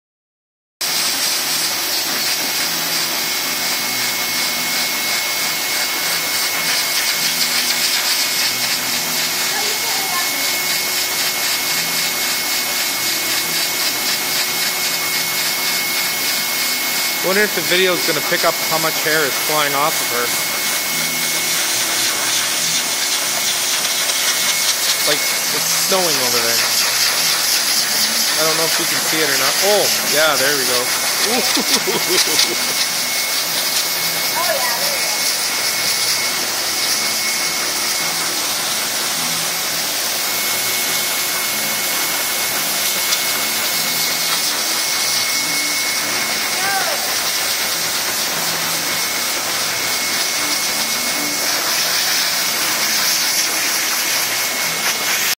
Blowing out Kiahs winter coat
Its like it is snowing in here. It is amazing how much dead fur is under there.